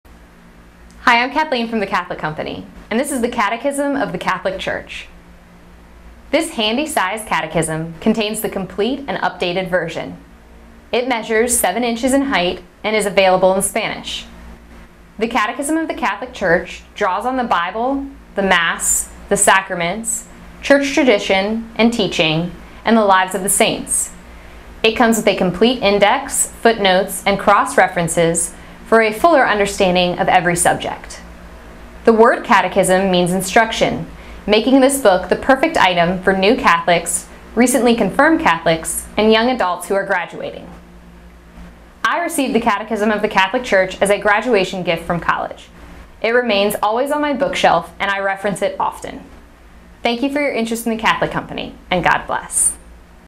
Catechism of the Catholic Church | The Catholic Company
To purchase the Catechism of the Catholic Church from The Catholic Company,

The Catechism of the Catholic Church is the normative resource for lay Catholic faithful to learn more about their faith.  The Catechism is organized in such a way to make looking up specific topics a breeze.  A great handbook and reference book when you need to research a particular topic about the Church, Faith, Bible, or morals.  This is an affordable version that is also available in Spanish.

To view The Catholic Company's entire selection of Catholic catechisms and guides,